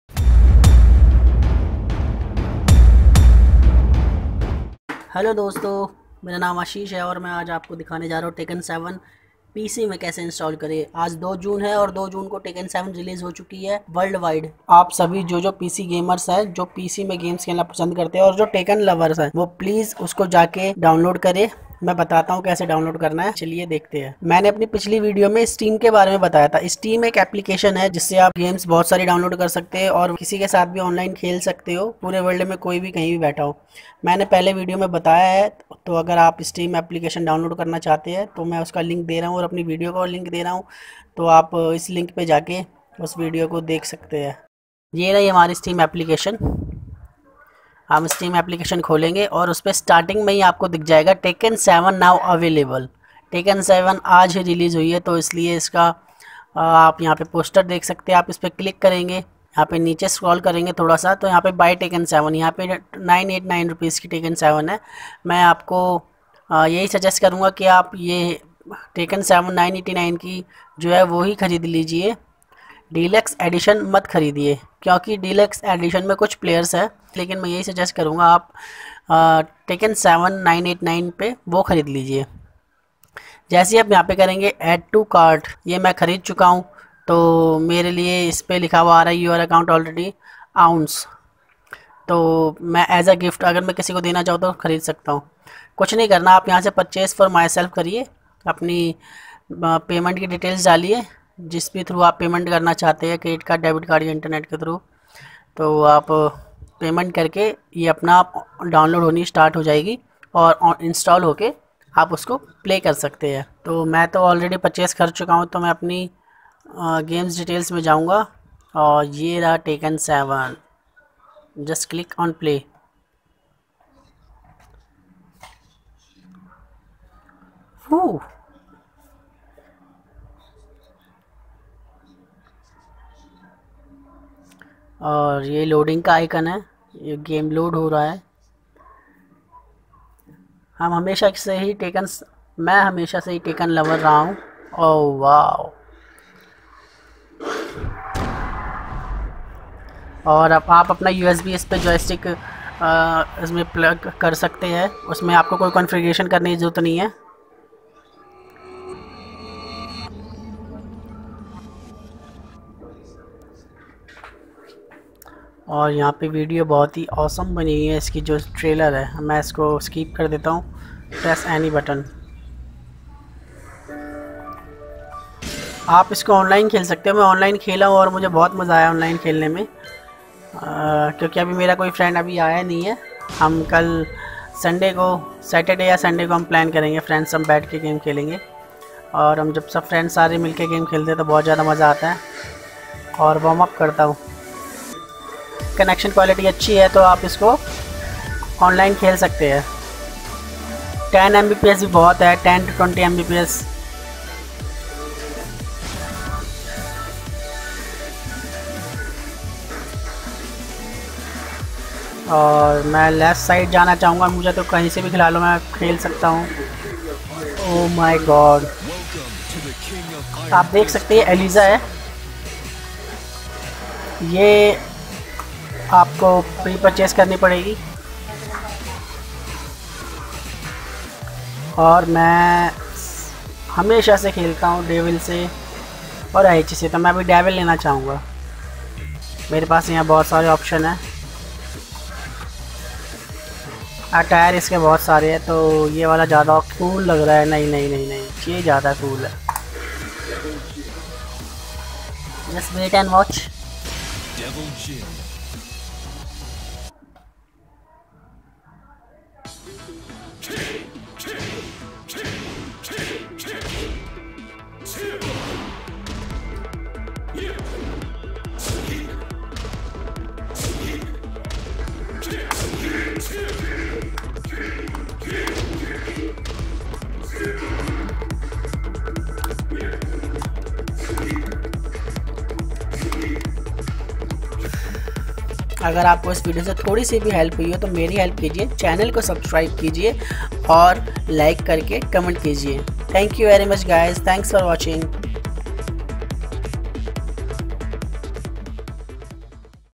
How to Download Tekken 7 PC from steam in HINDI.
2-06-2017 is the releasing date of Tekken 7 PC,  In this video i am telling that how to install Tekken 7 PC in Hindi.